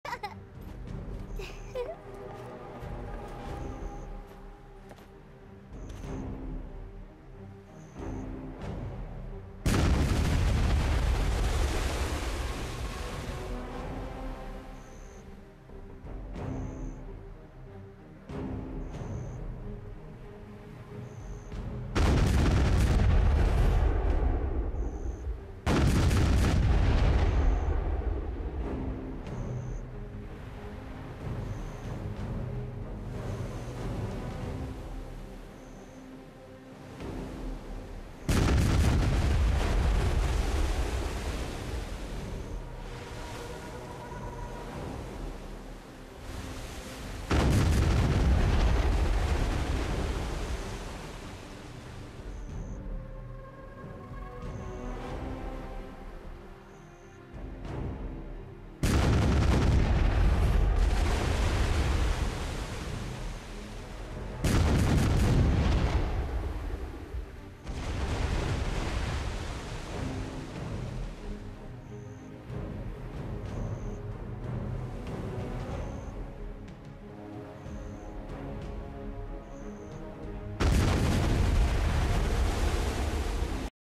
New Cannon Shooting Method
This Global Lab Update 2 April 2020
"Precision shelling mode" for Improved Sailboat, Improved Frigate, Caravel, Galleass, Carrack
- Press "Q" or "E" for take camera aim
- LMB or RMB for shooting cannon
- RAM skill key has move to "F" key